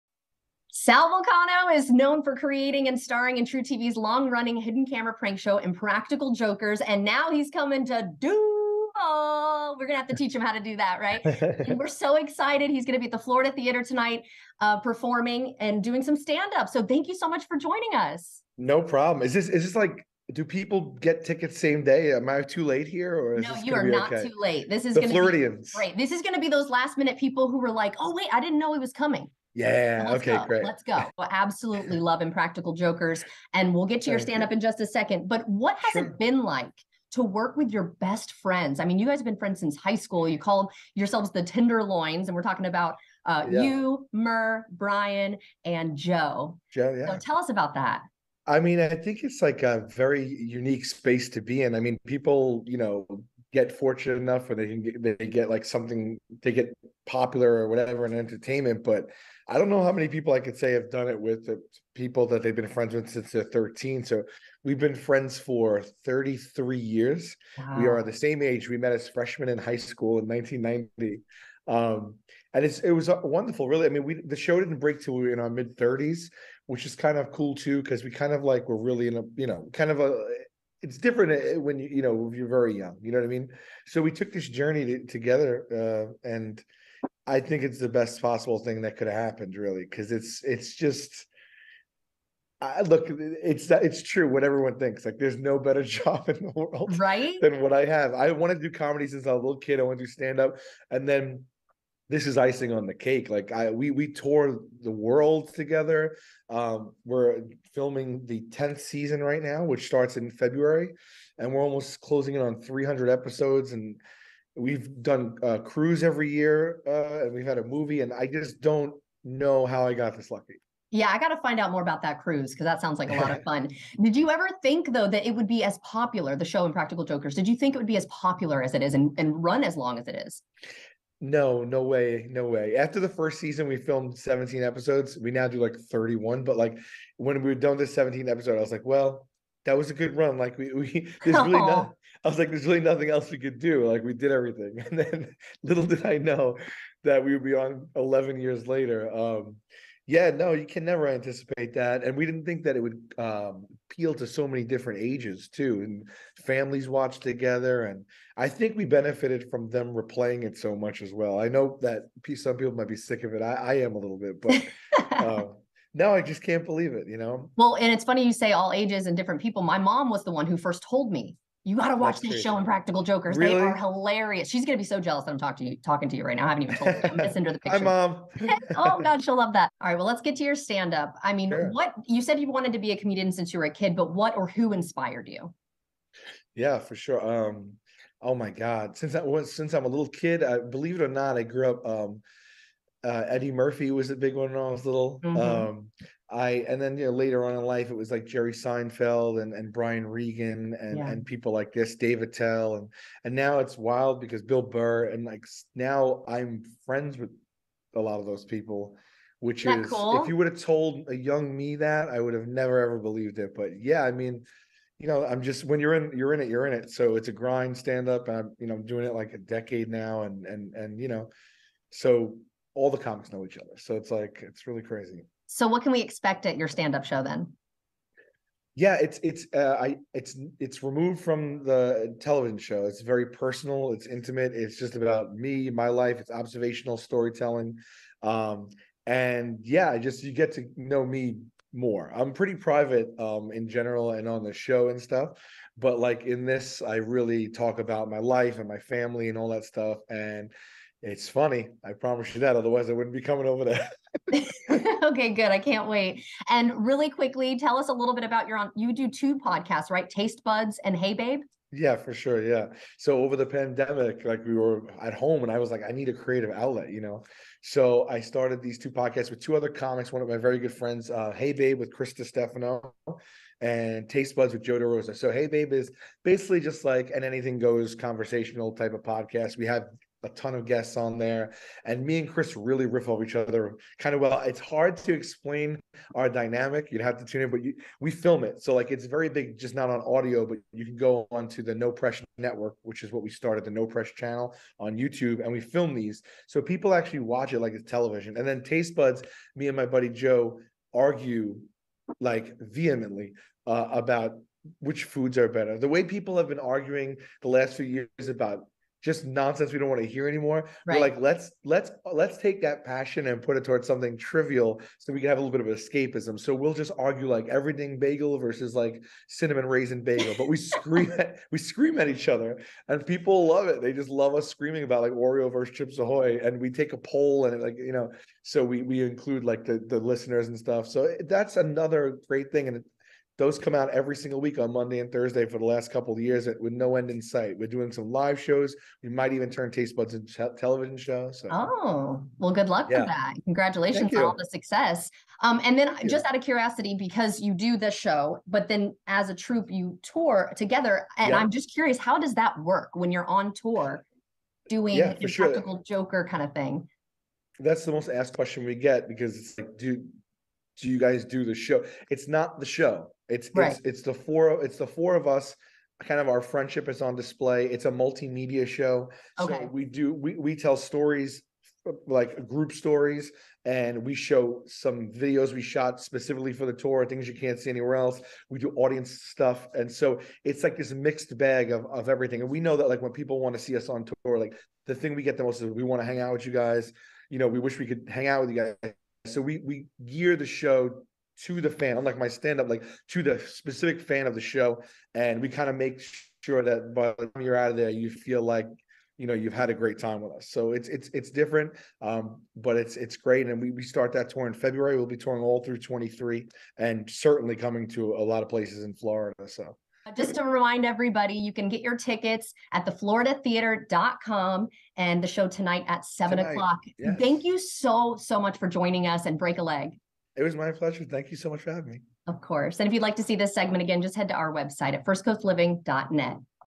Impractical Joker, Sal Vulcano is heading to Jacksonville!
You still have time to purchase your tickets to see him tonight at The Florida Theatre.